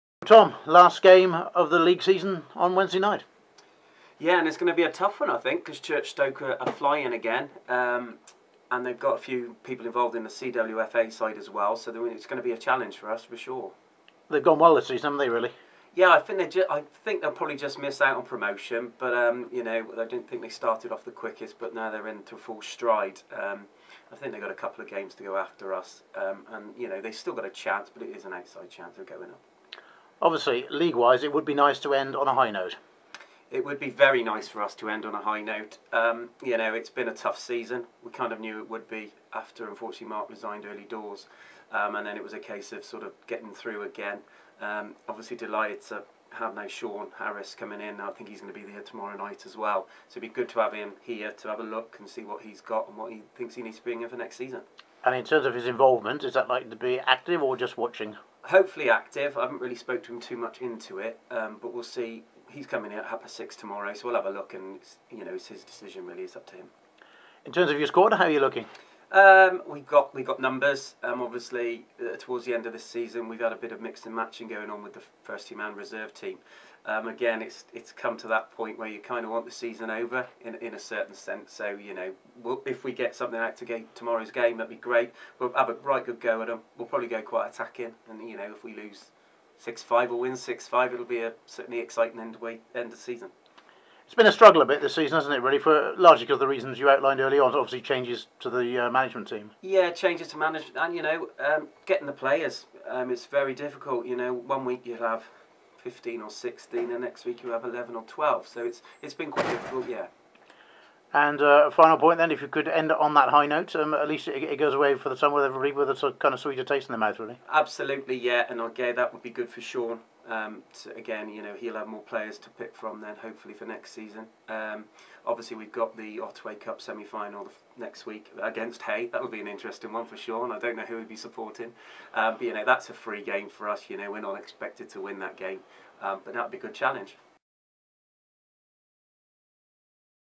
tom ammonds pre churchstoke home 040417
Presteigne St Andrews FC's manager Tom Ammonds looks ahead to his final game in charge of the first team in which they will host Churchstoke in a Spar Mid Wales League, second-division game at Llanandras Park on Wednesday, 5 April 2017.